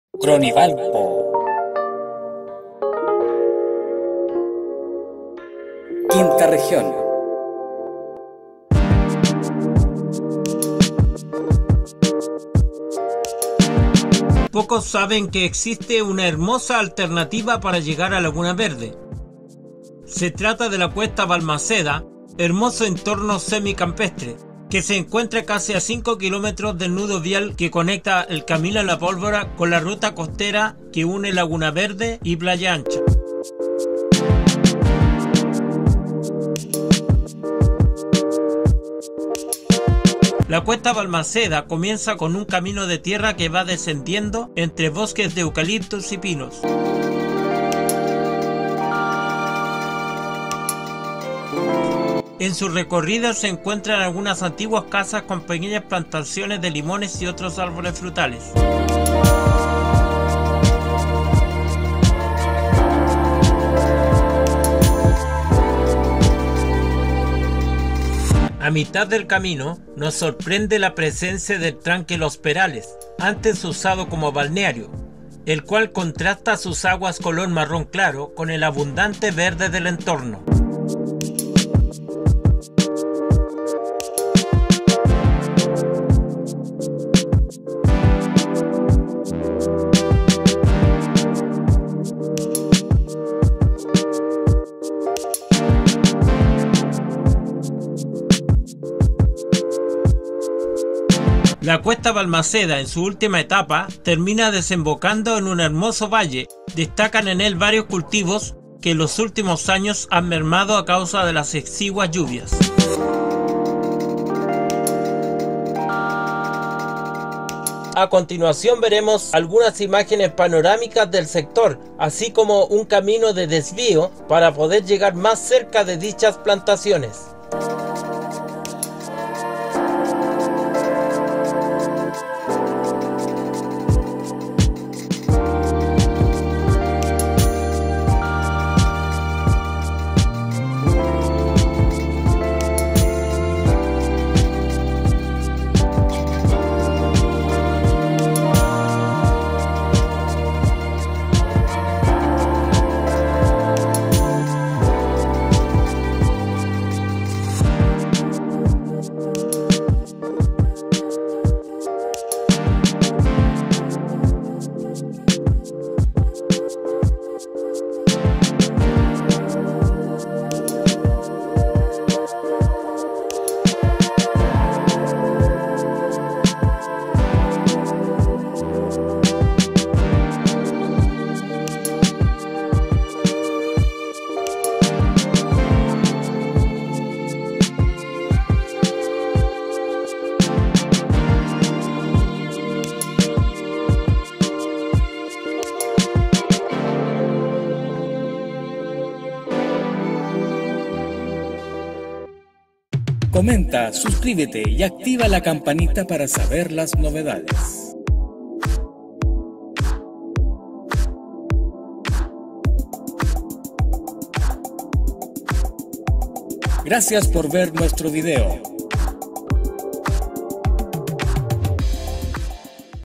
Hermoso Campo Escondido Detrás de Valparaiso y Laguna Verde
Un sector semi rural, poco conocido y ruta alternativa, para llegar a Laguna Verde, en Valparaiso.